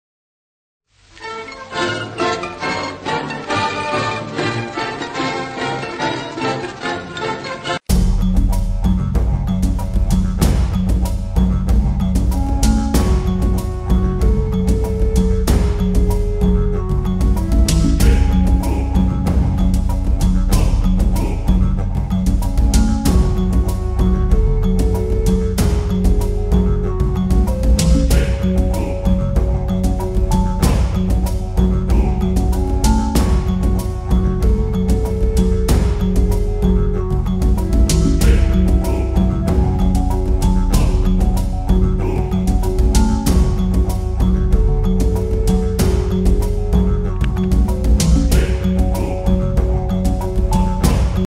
Twiggy & 'The Stick'
Stick Insects.. They're ideal pets, the food is free from the garden, they require very little exercise and they can perform tricks like 'stay', 'don't move' and .. well, that's it!